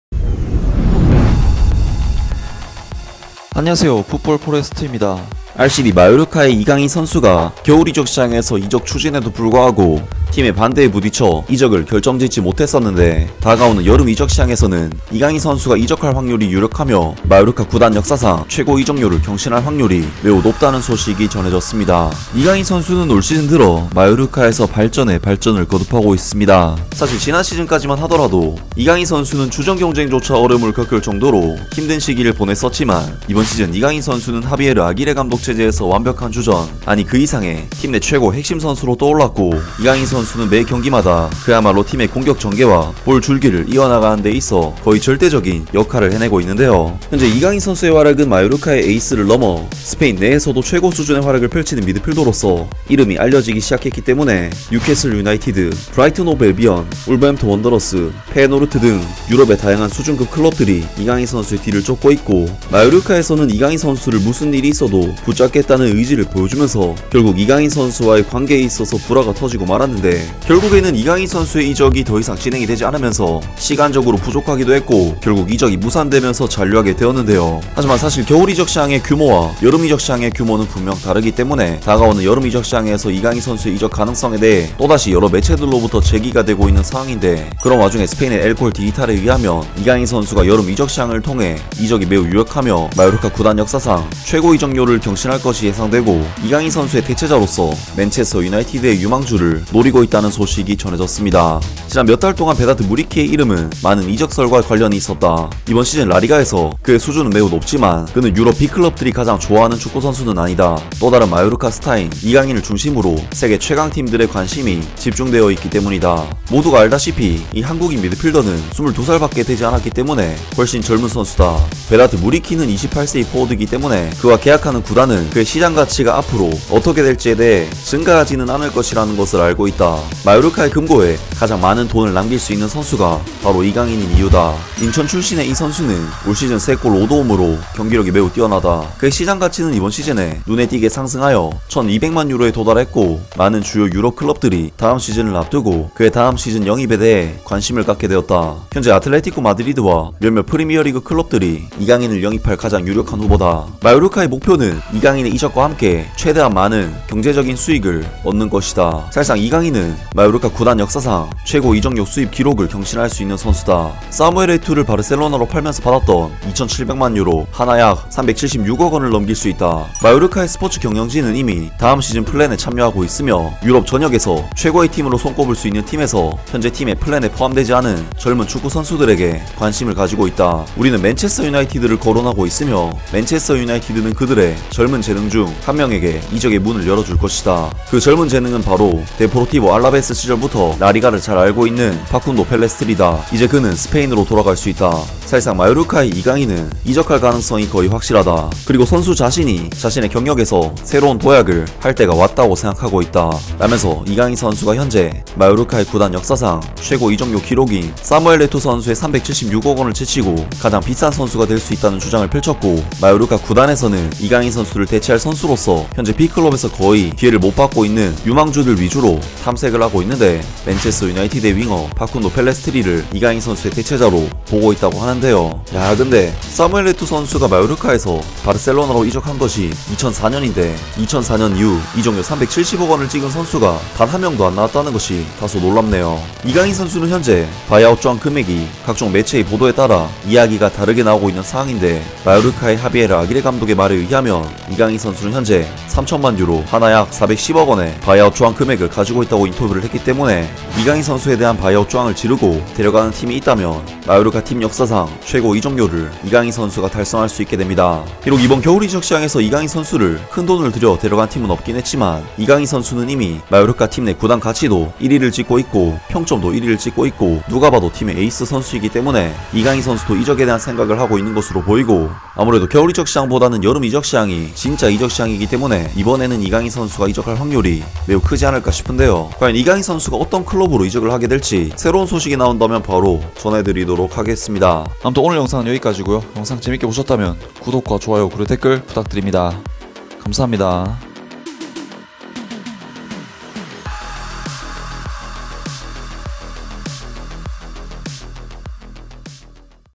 "이강인 마요르카 이적료 신기록 달성한다" 이강인 여름 이적 유력, 마요르카 이강인 대체자로 맨유 윙어 찾았다!
구독!